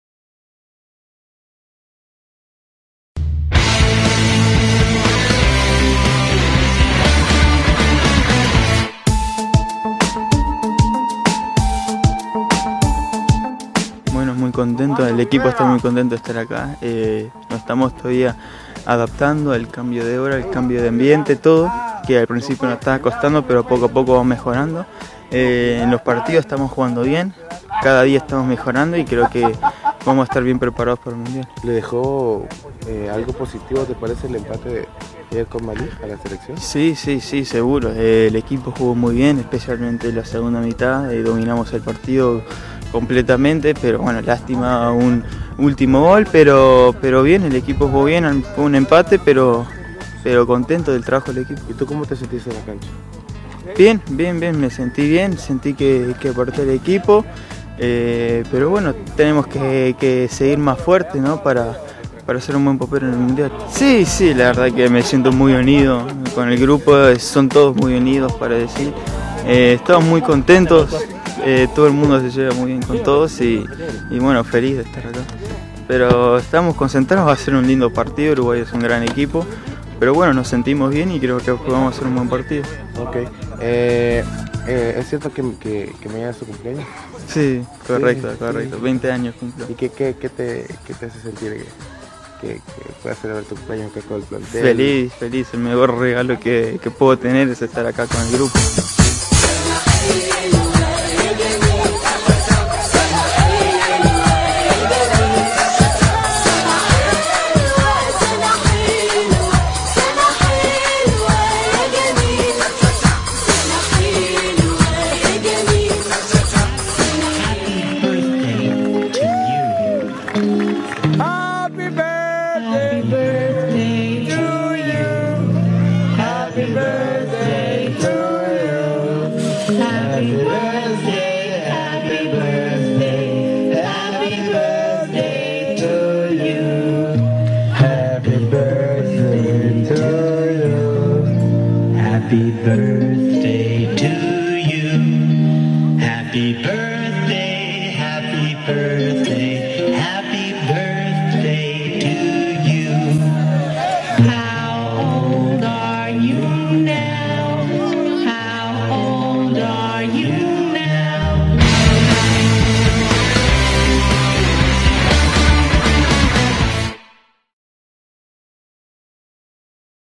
Celebracion del cumpleaños de Tomás Granitto reportaje de El Grafico/La Prensa Grafica
Seleccion Sub20 de futbol de El Salvador    12/06/2013